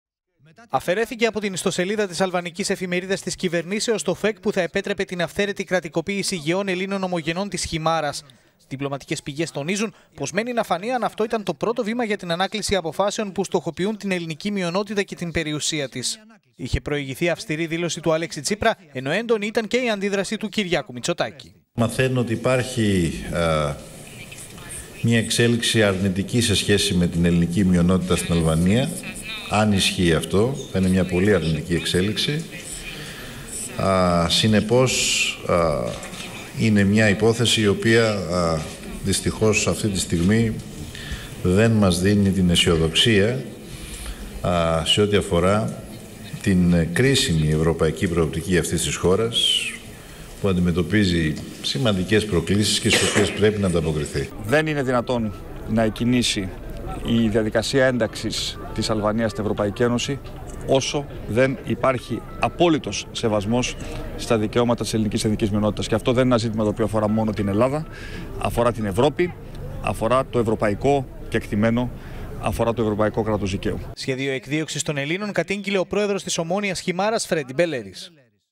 Αφαιρέθηκε το αλβανικό ΦΕΚ για τη δήμευση των περιουσιών των Ελλήνων
Διπλωματικές πηγές τονίζουν ότι ήταν ιδιαίτερα σαφής και η τοποθέτηση του πρωθυπουργού, η οποία δεν άφησε κανένα περιθώριο παρερμηνείας από την αλβανική ηγεσία…